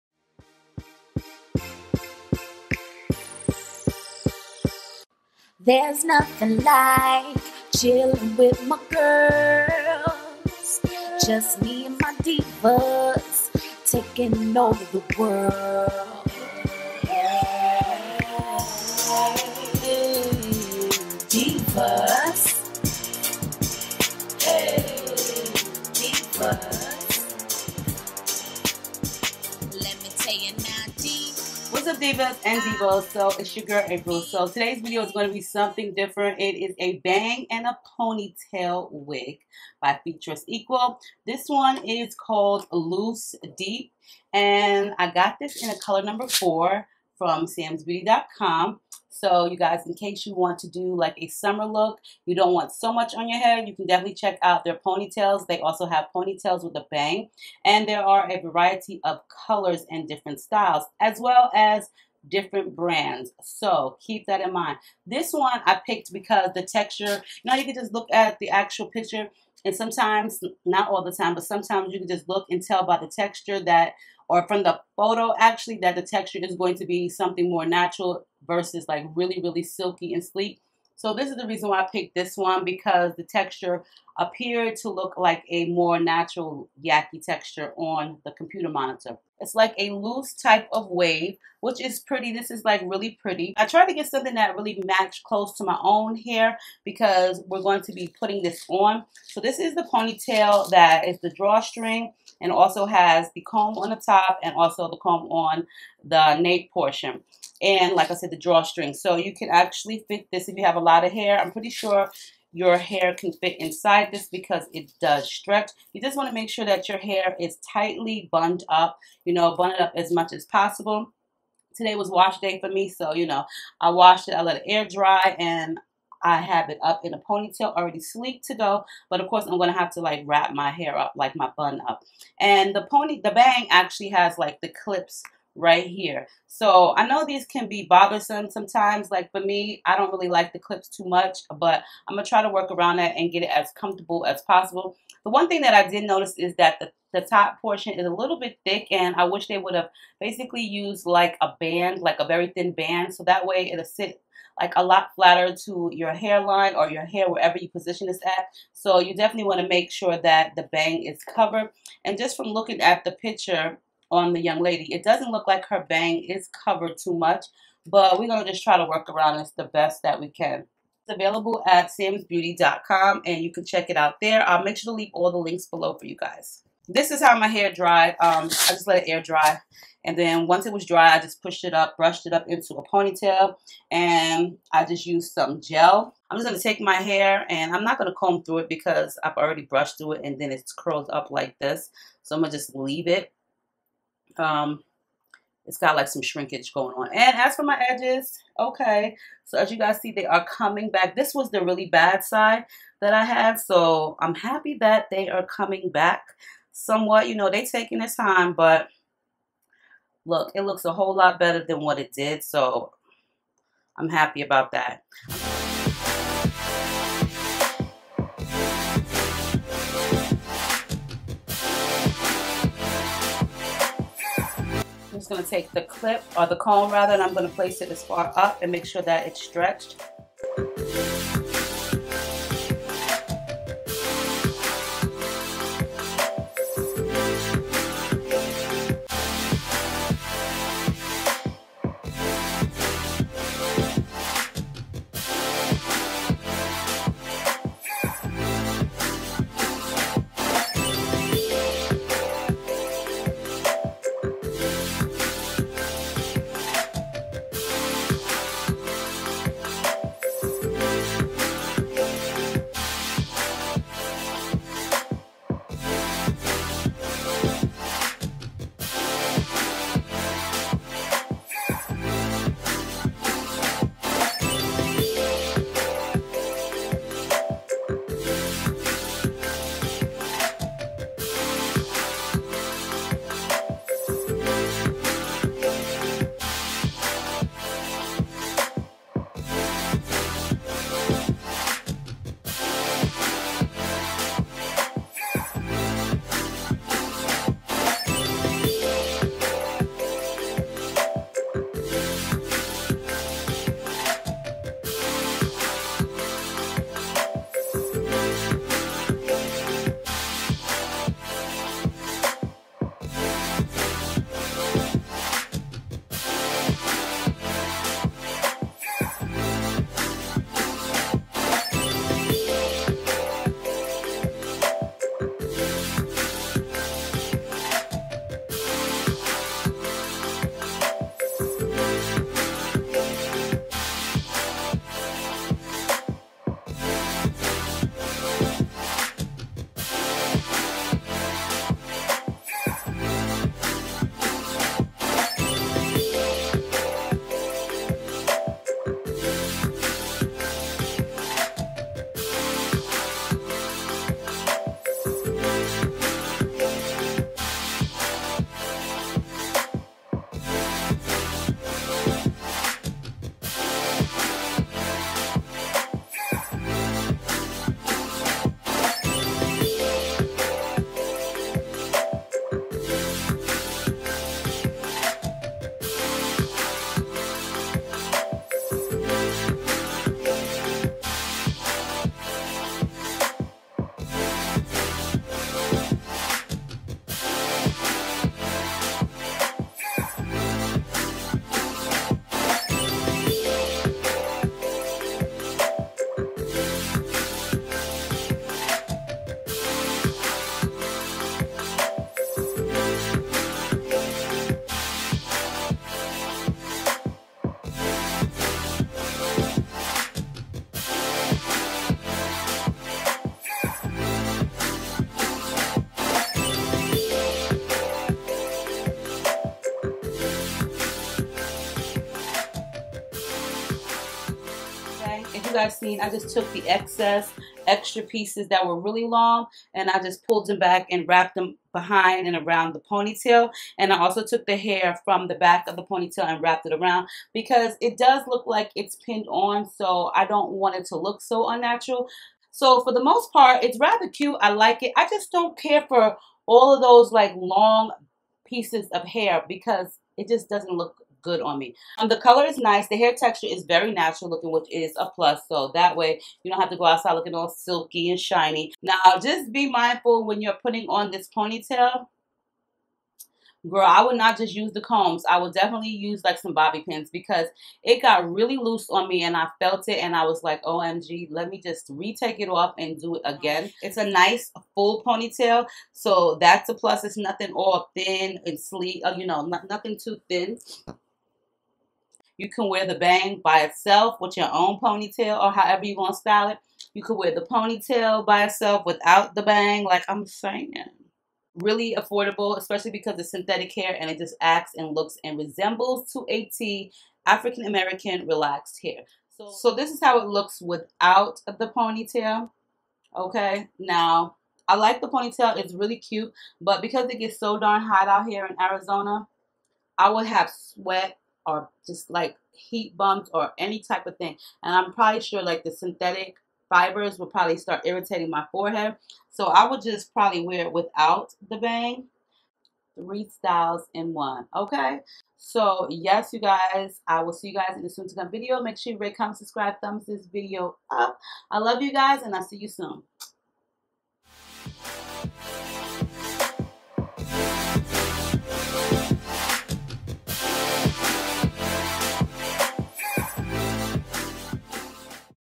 EASY INSTANT PONYTAIL ⎮ FREETRESS EQUAL SYNTHETIC HAIR PONYTAIL & CHINA BANG Samsbeauty.com
A great Summer Hairstyle. AFFORDABLE & Easy in instants a ponytail & China Bang all by Freetress Equal. Synthetic hair beginner friendly, fashionable & easy to wear. Add baby hair or wear the ponytail separate as well as the bang available at Samsbeauty.com

WRITE OR SEND US LOVE GIFTS TO:
Muffinismylovers

SOME OF MY FAVORITE SITES